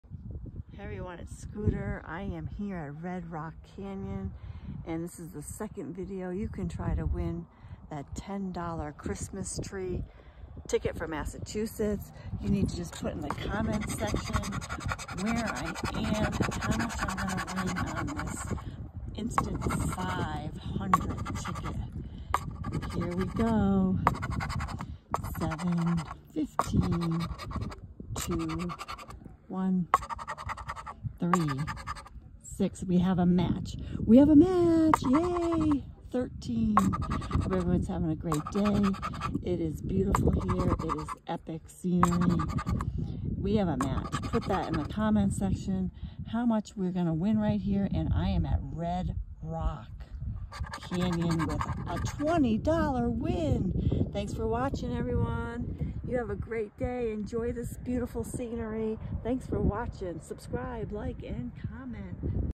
Where is Scooter? Finale chance to win Massachusetts $10 Christmas ticket. 💕
Last chance to comment on “Where is Scooter”
3rd post to have your name on the spin wheel for placing your comment with location & winnings. Your Name will go on a spin wheel to win the $10 Massachusetts holiday tree ticket.